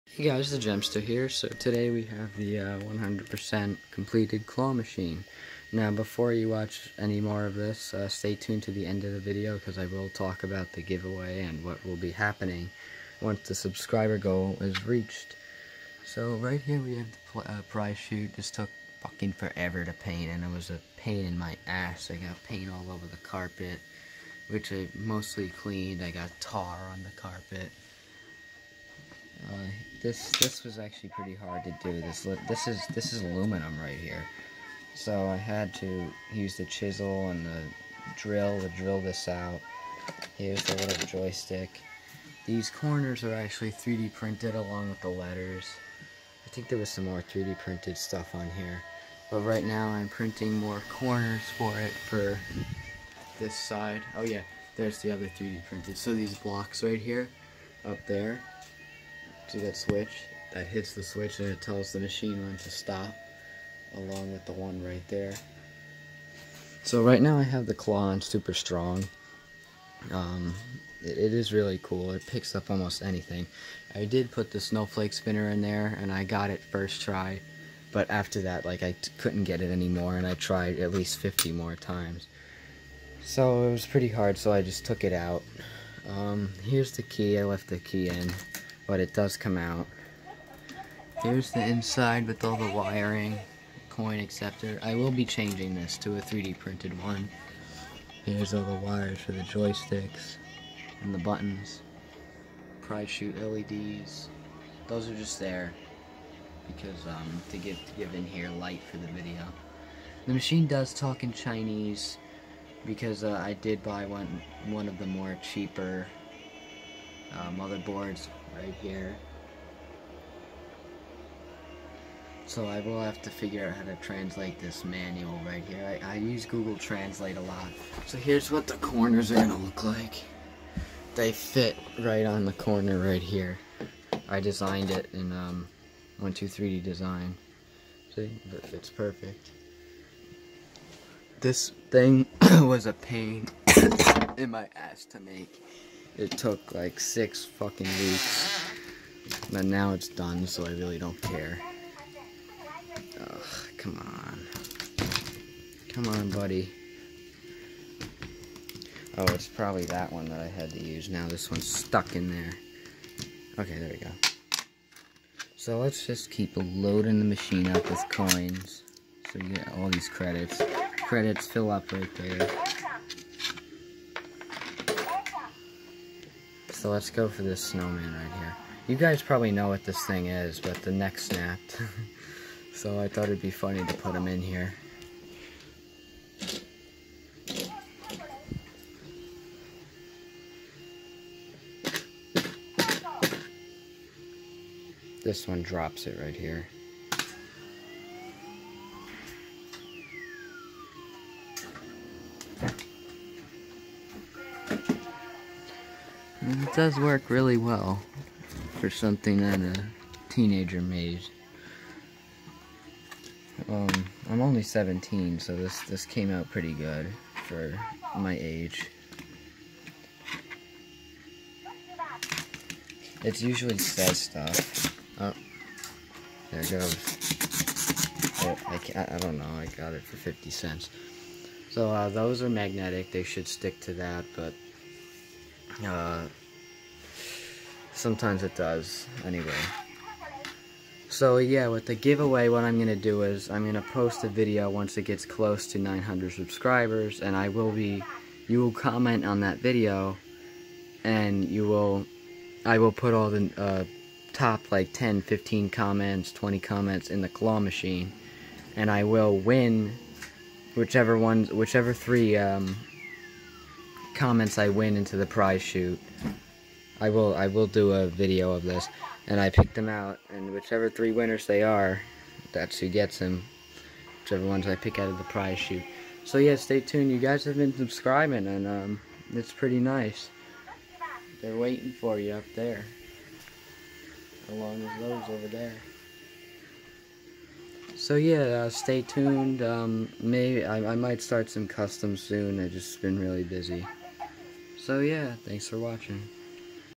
GIVE AWAY UPDATE + Completed DIY Claw Machine!
After weeks of work, here is the final result!

Also, Connect Here!

 @Gemmy Industries ​